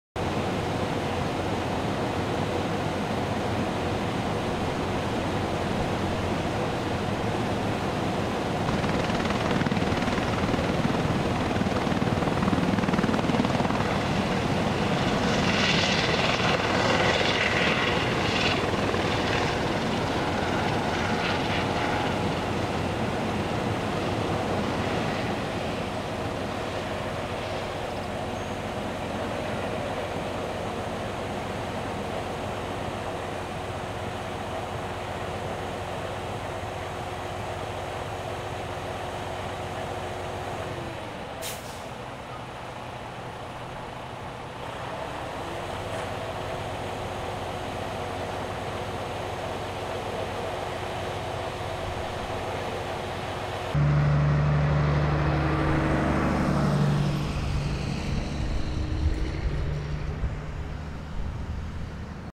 Man Falls From Scaffolding
Victim Air-Lifted by Medical Helicopter from Ginger Cove